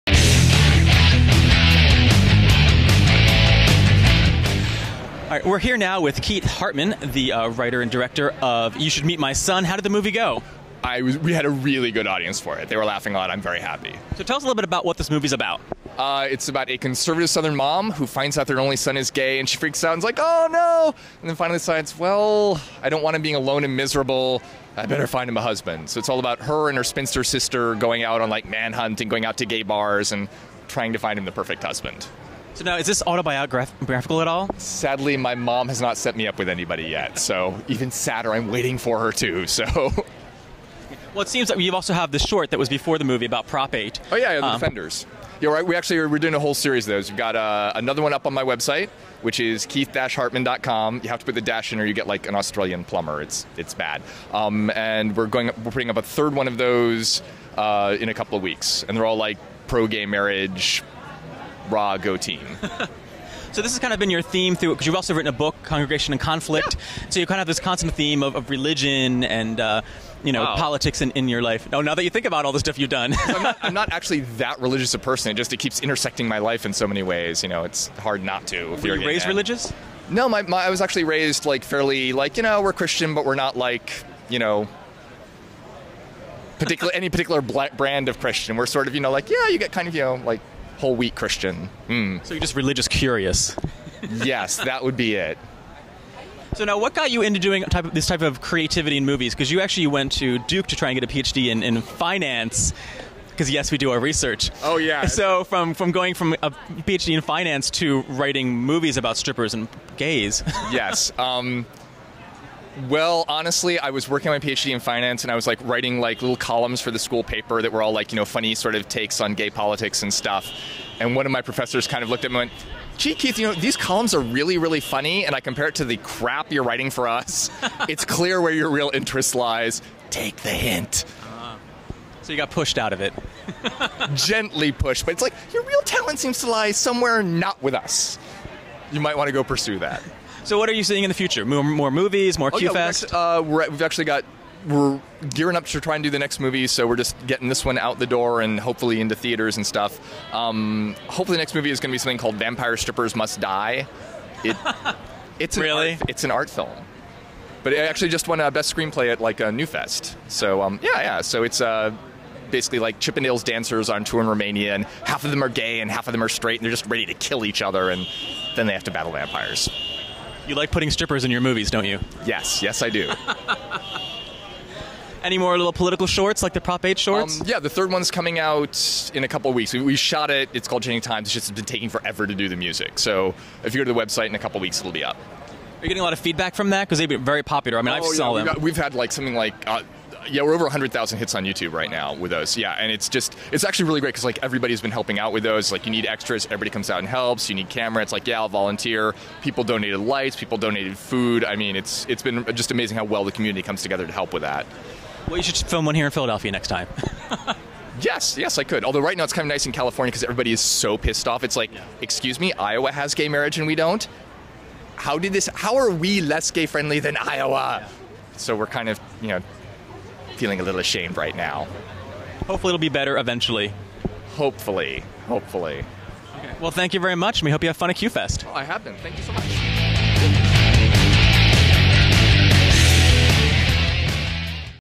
QFest 2010 - You Should Meet My Son!
PhillyGayCalendar.com interviews Keith Hartman, director and writer of "You Should Meet My Son!" at QFest 2010 in Philadelphia.